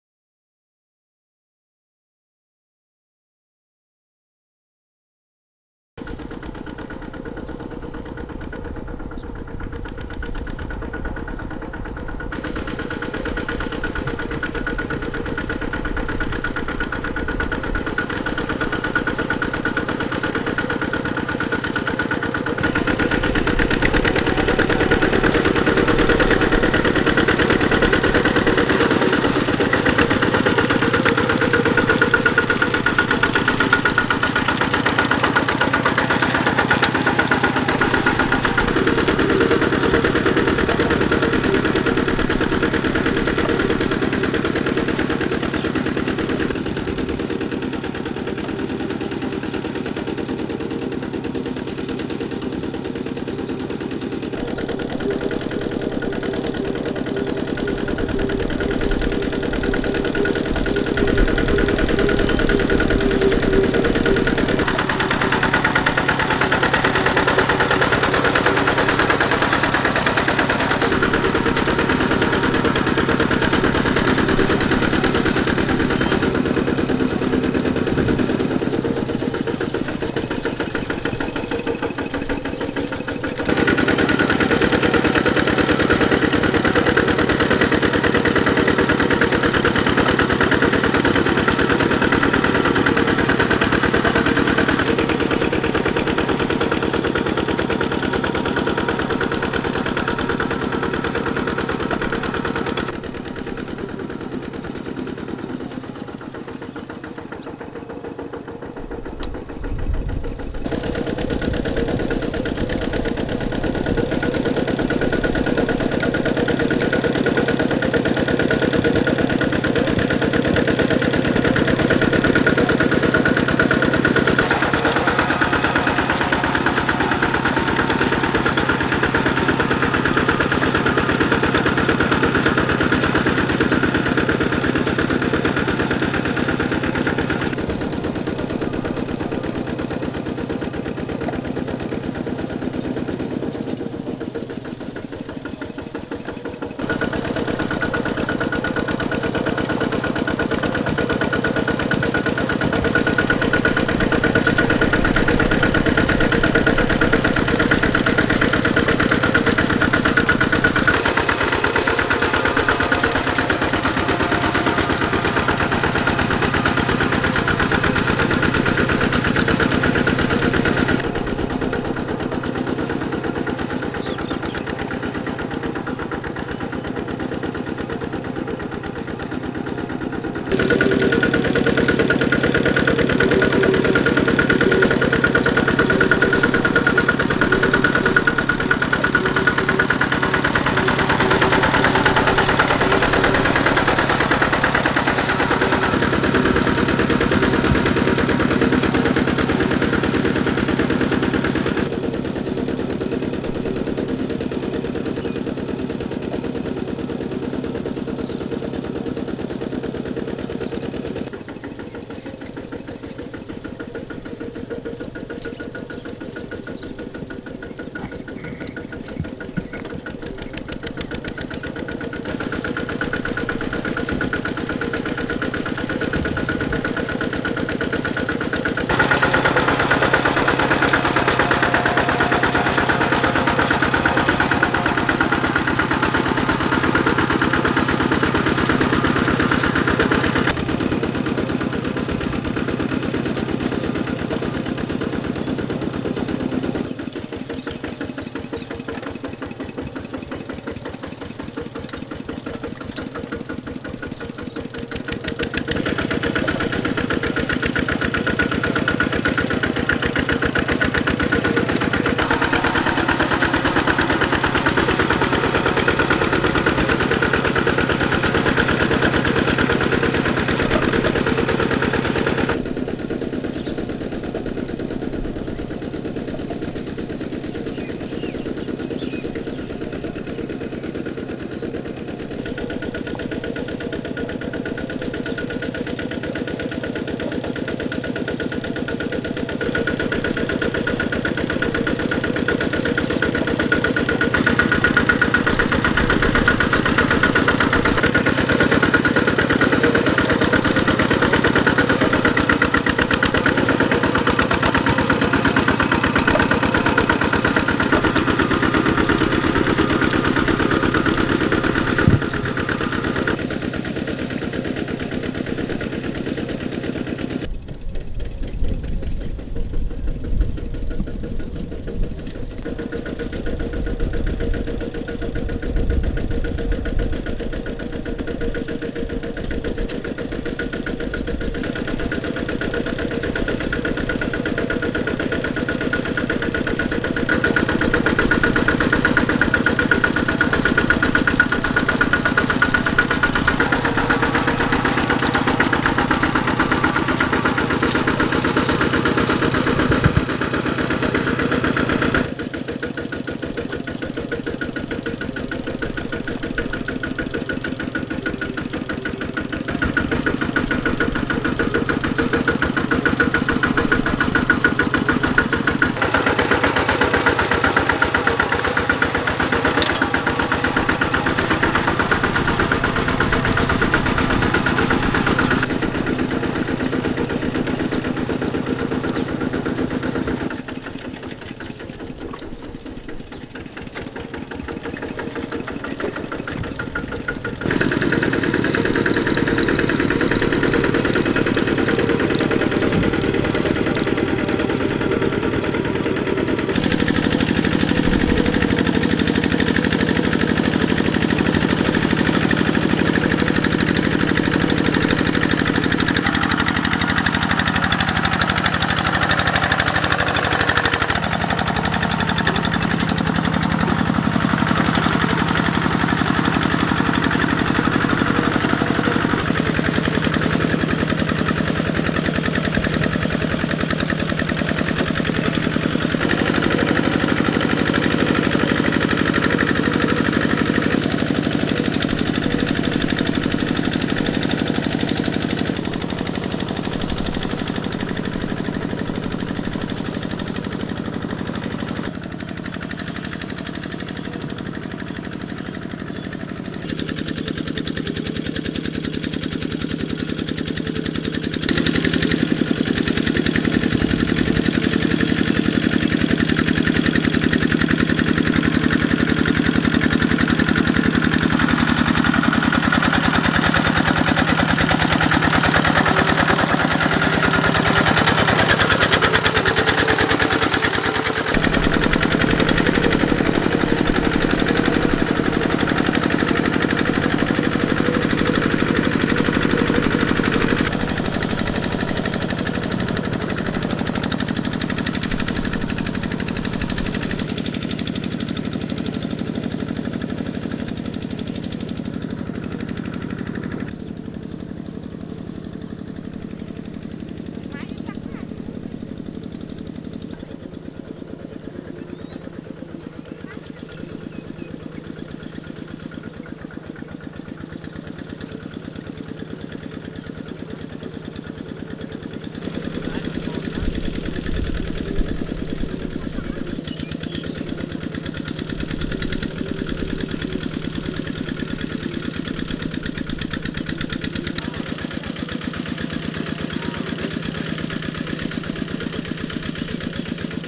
Small Tractor Kubota Plowing field | Amazing small tractor kubota plowing field
Tractor Khmer Farmer,គោយន្តភ្ជួរស្រែ,Kubota Cambodia,
ប្រជាជនកម្ពុជាបានផ្លាស់ប្តូរការធ្វើស្រែដោយប្រើកម្លាំងគោមកប្រើគោយន្តវិញដើម្បីបានចំនេញពេលវេលានិងកម្លាំងរបស់កសិករ
Tractor Kubota
Tractor Cambodia,
Cambodia Farmer,